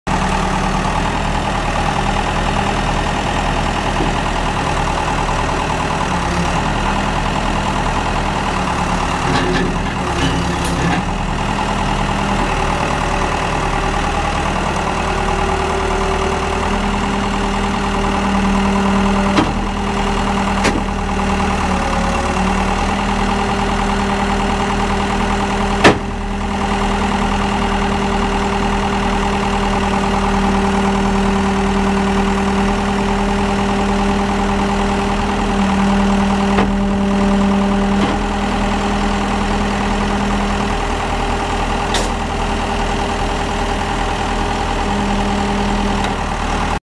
Case 570MXT Wheel Loader w/Box Blade at Public Auction
w/Enclosed Cab, Radio, 4 Cylinder Diesel Automatic A/C Sn# JJG0380541 Hours 02233
This item will be sold to the highest bidder at no reserve on 10/10/15.